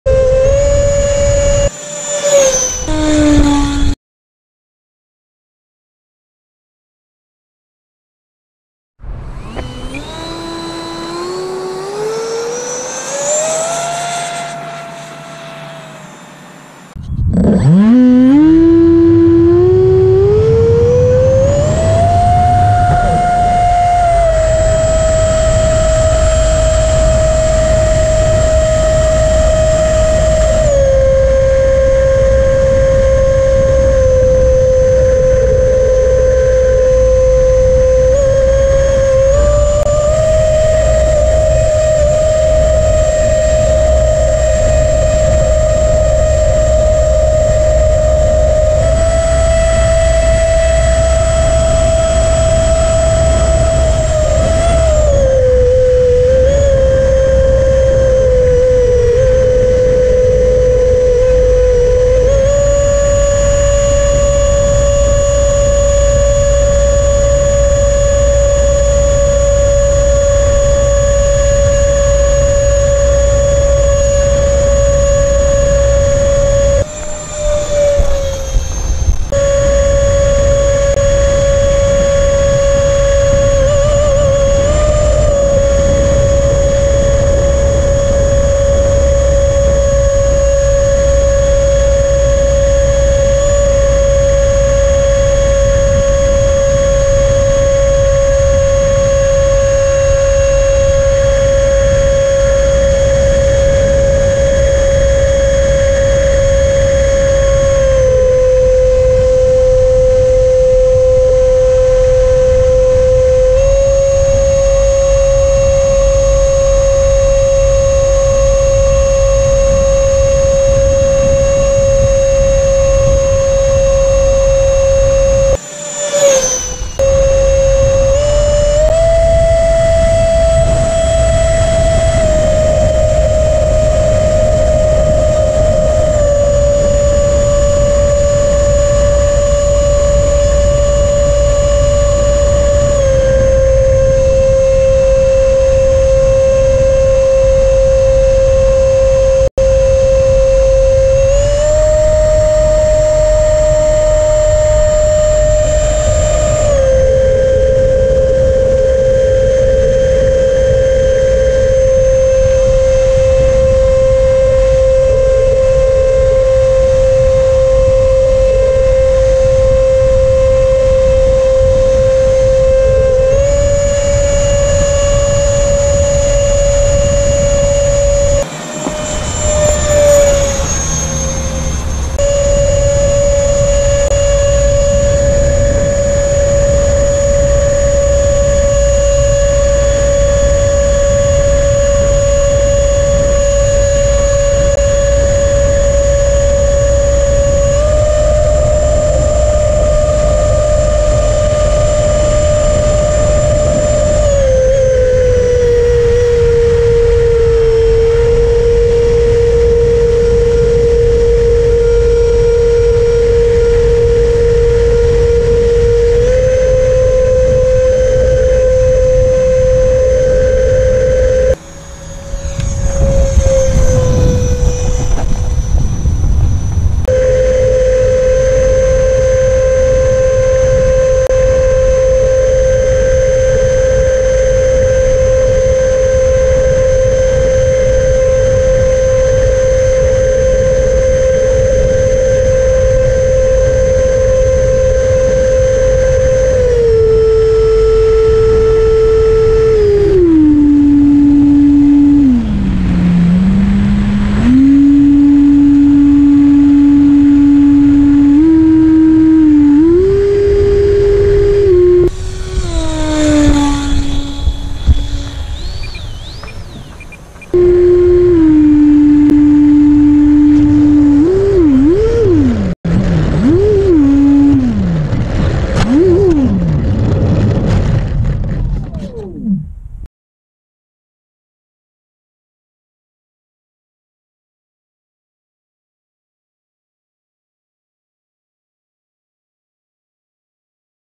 Freewing STINGER 64mm 4S EDF Jet FLIGHT DEMO - - FARM SPRINKLER STRAFING! (GoPro onboard)
Freewing STINGER 64mm 4S EDF Jet FLIGHT DEMO -- FARM SPRINKLER STRAFING (GoPro onboard).  Freewing FJ10412P FJ10421P FJ1041108, HobbyKing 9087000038